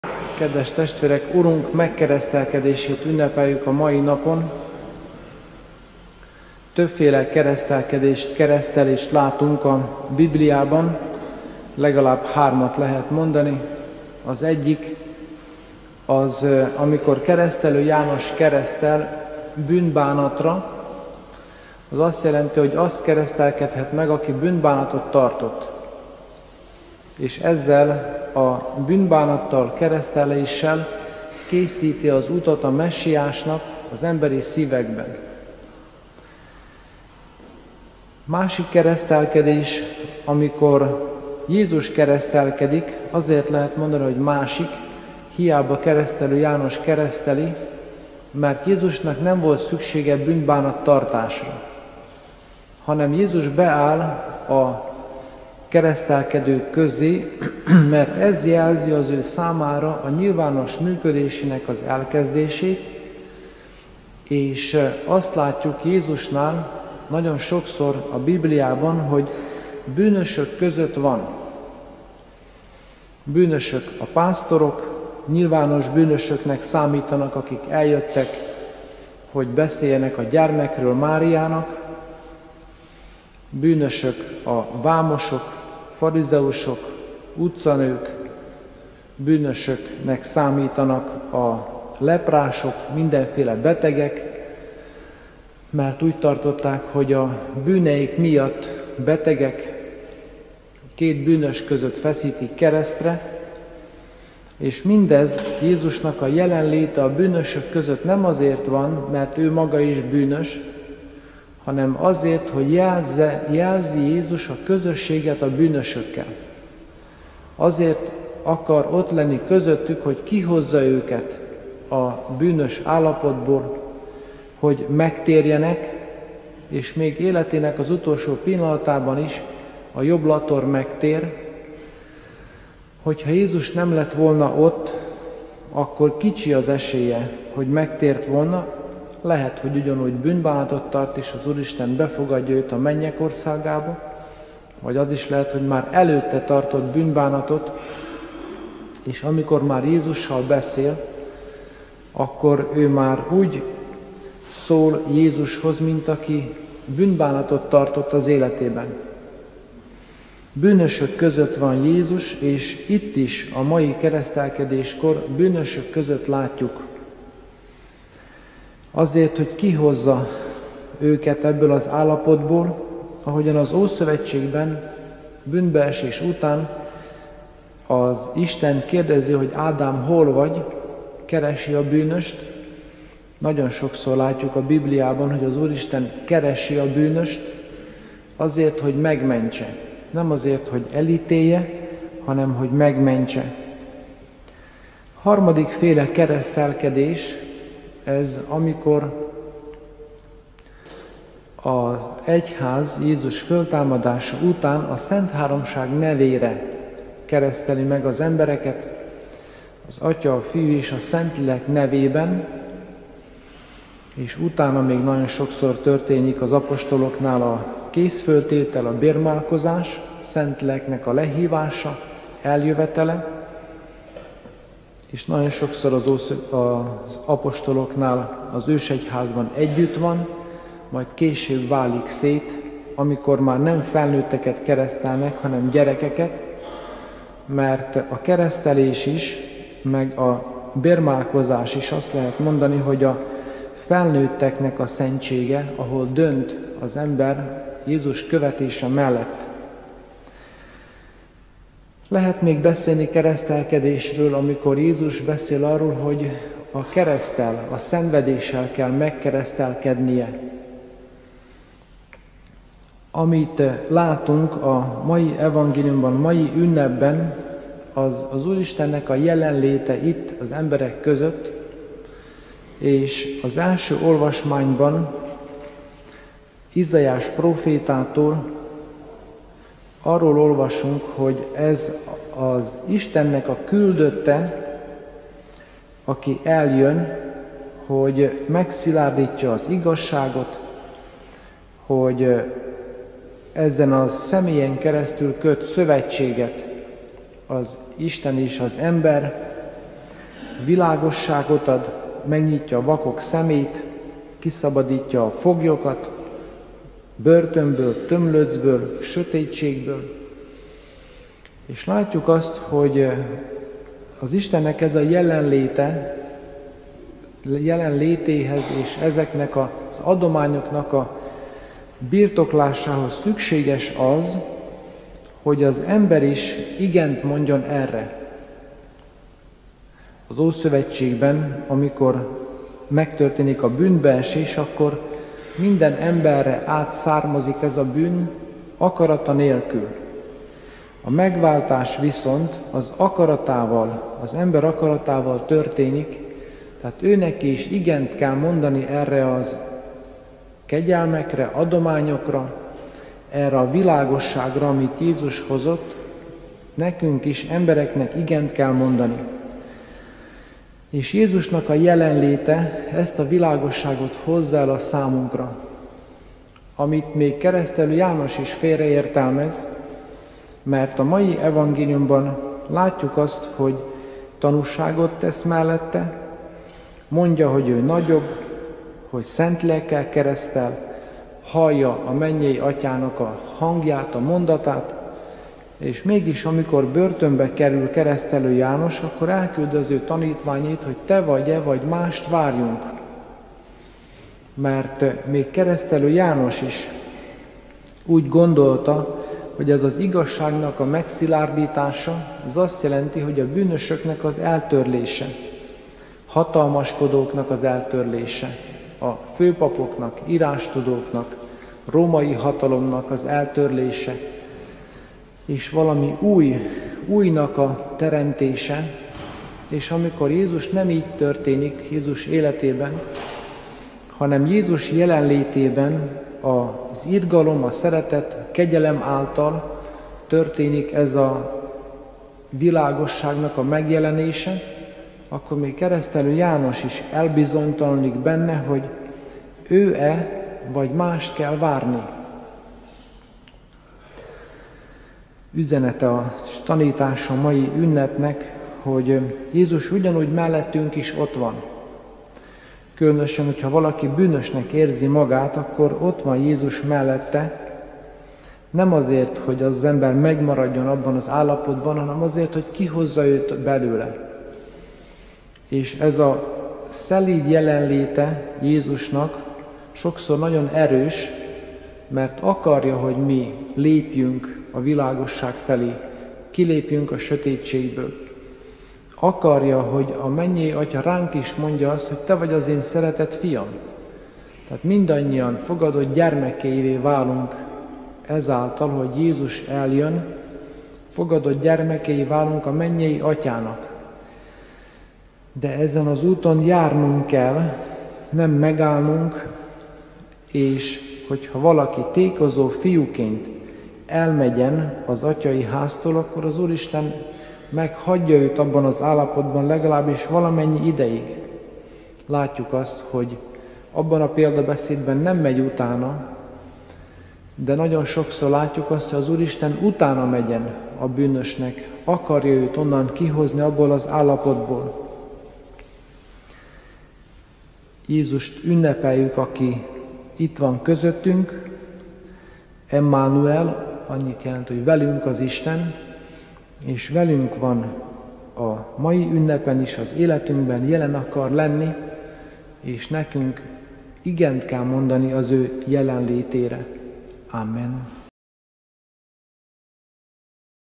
Igent mondani az Ő jelenlétére
📌Jèzus keresi a bűnöst.
📌A megváltás csak az ember akaratával történik.
📌Emánuel - velünk az Isten!

Prédikáció: Pogány István Munkácsi plébános